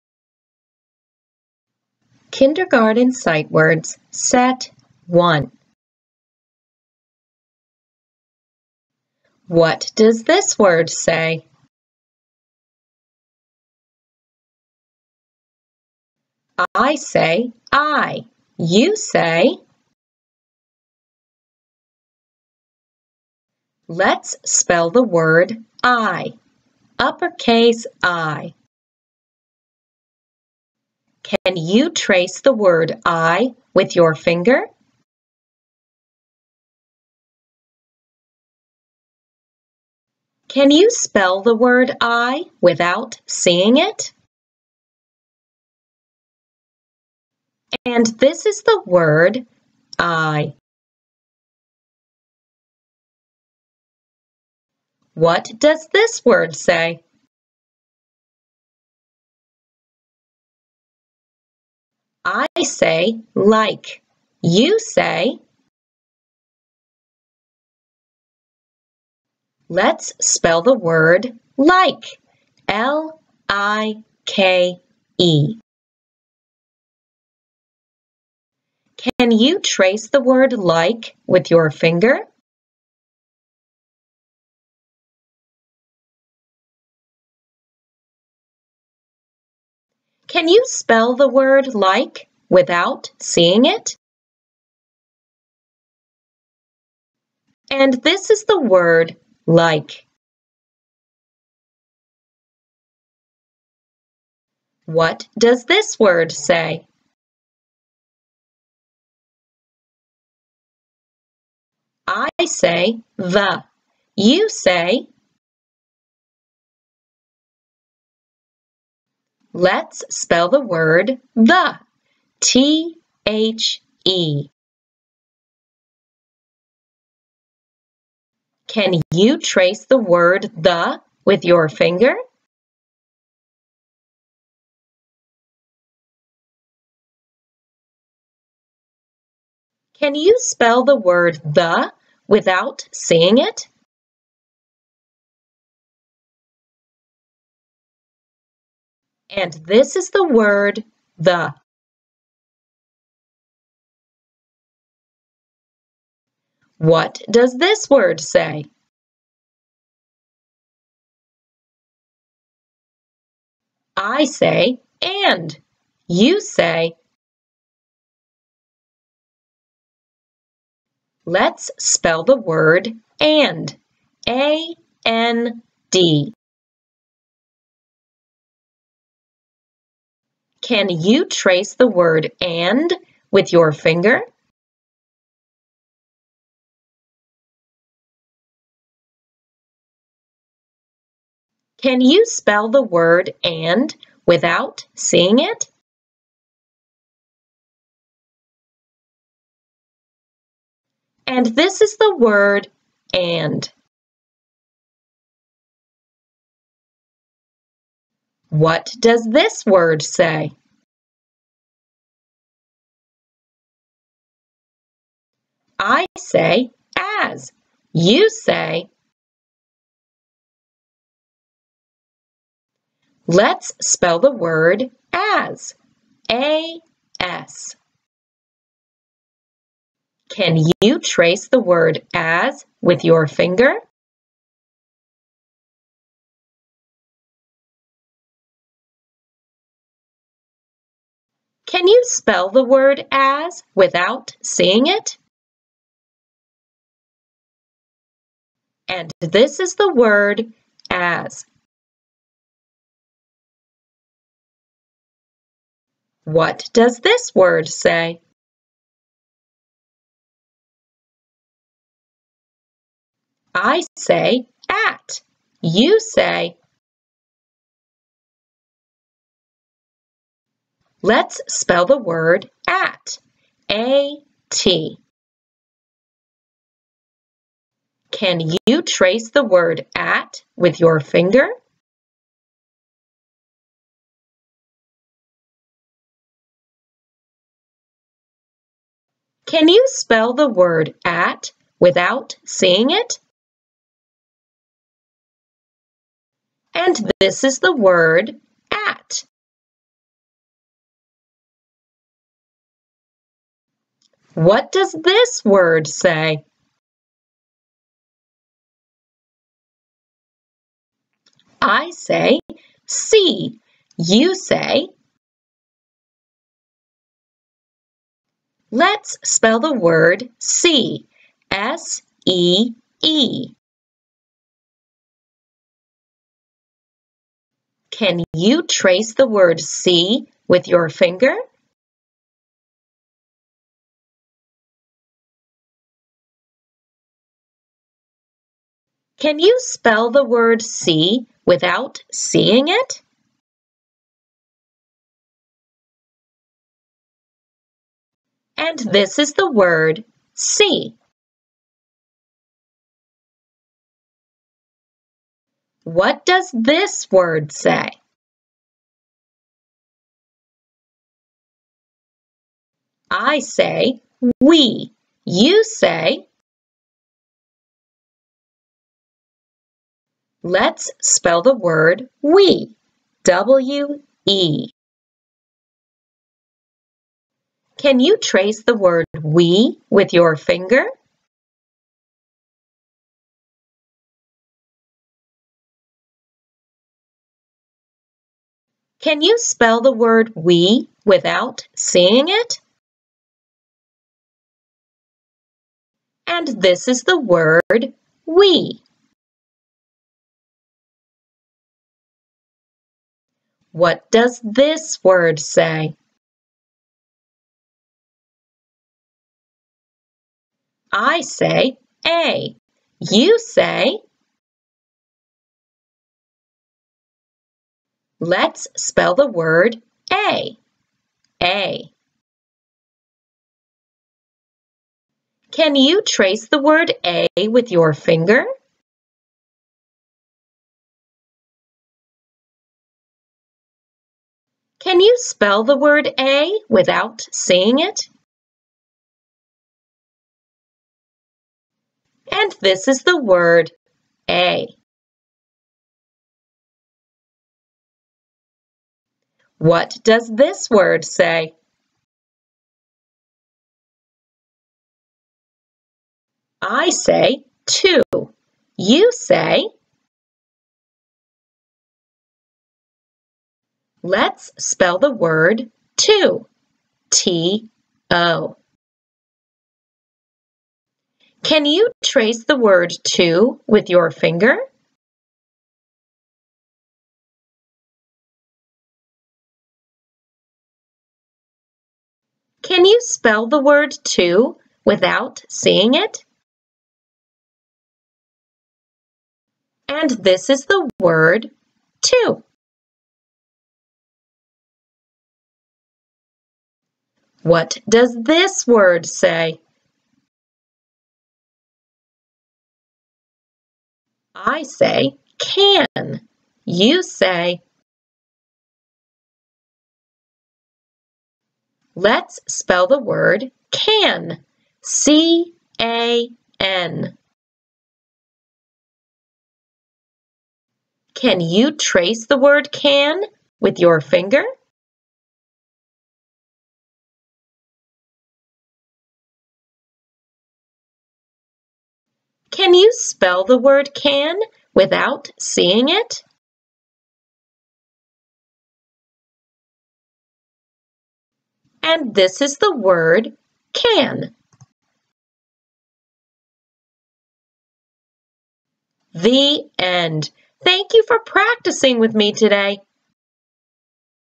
Kindergarten Fry Words Sight Word Movie 1 (Words 1-11)
This video helps students to independently practice the following sight words:  I, like, the, and, as, at, see, we, a, to, & can.